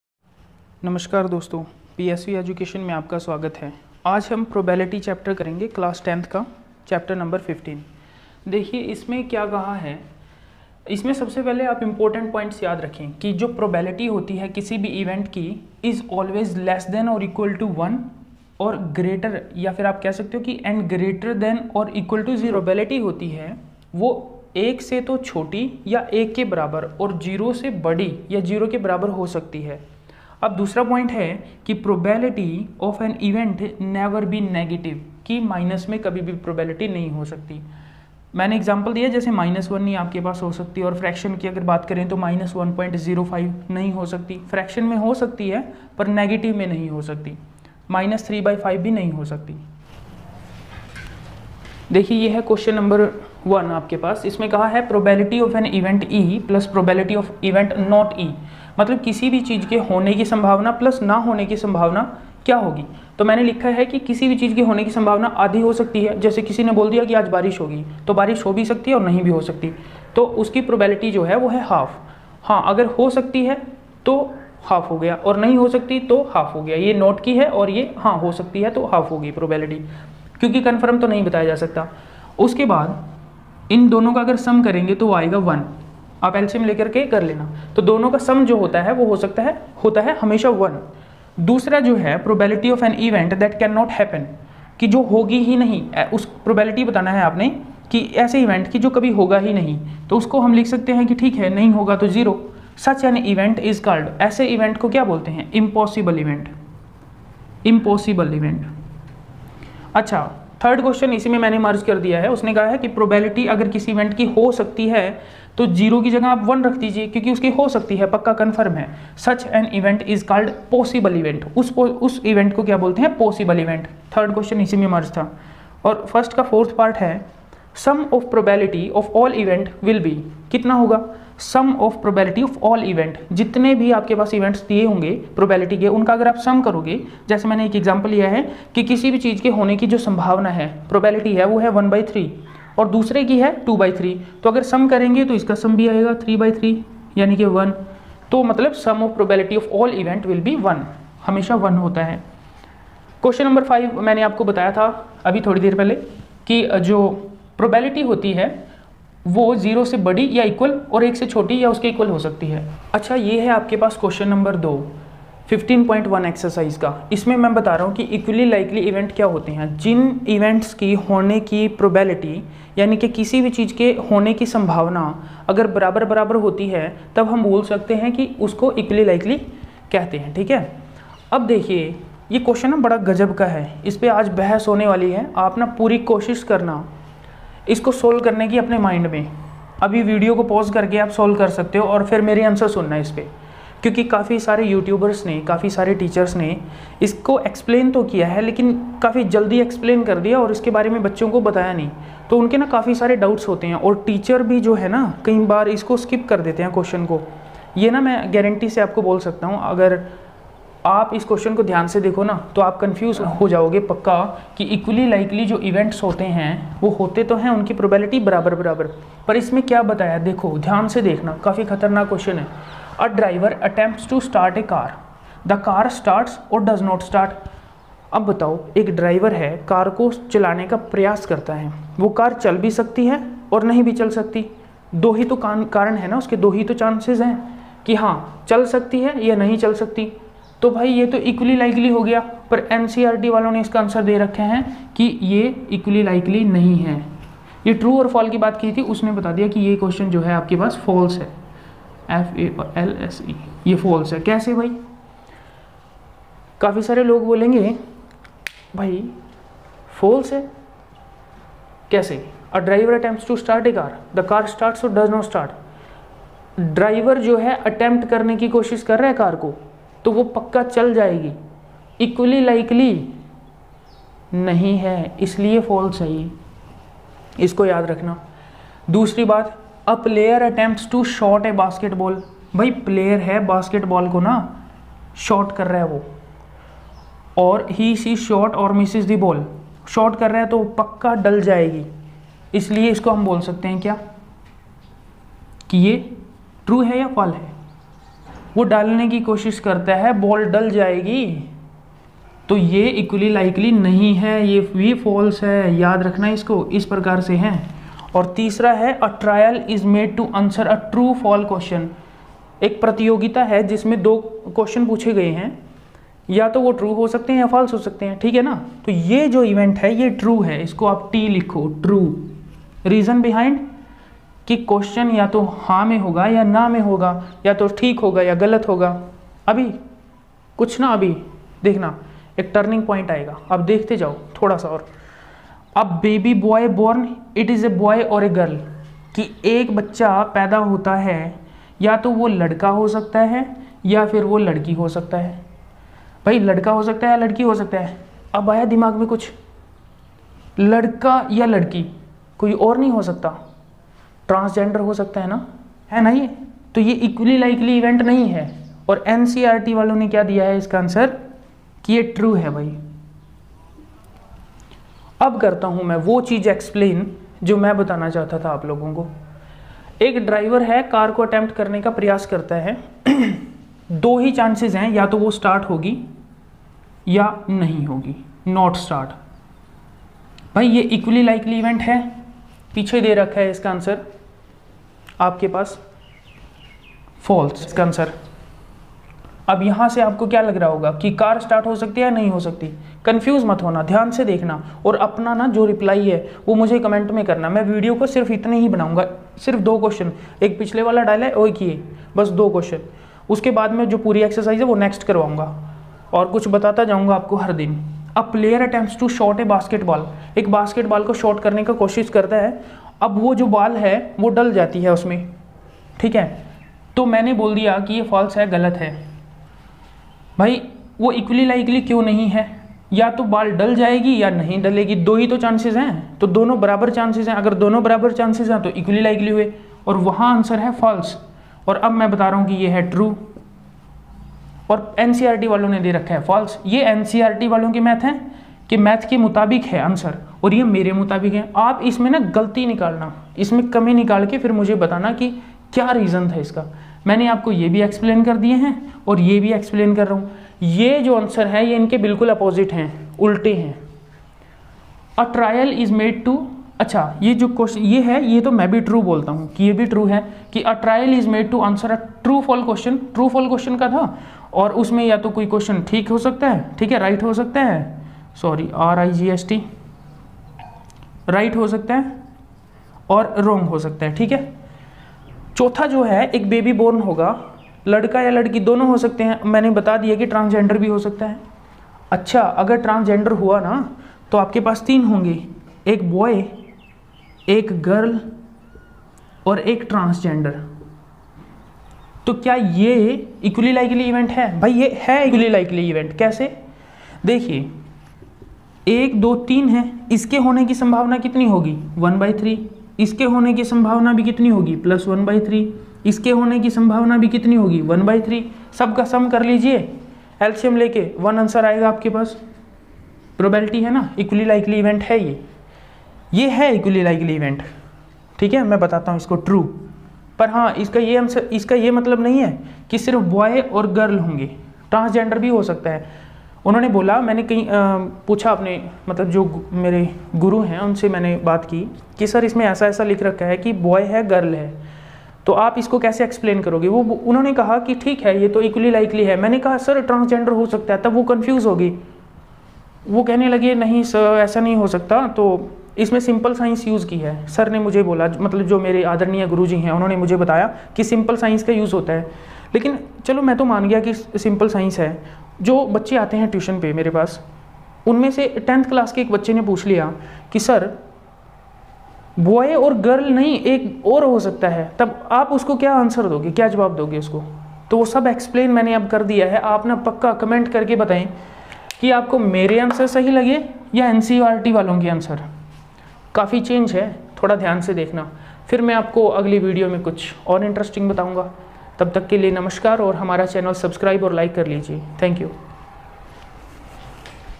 Probability / प्रायिकता confusing question | Class 10th exercise 15 Ncert solutions
Here in this video , i have explained one confusing question which looks very difficult for some students.

My Best Mic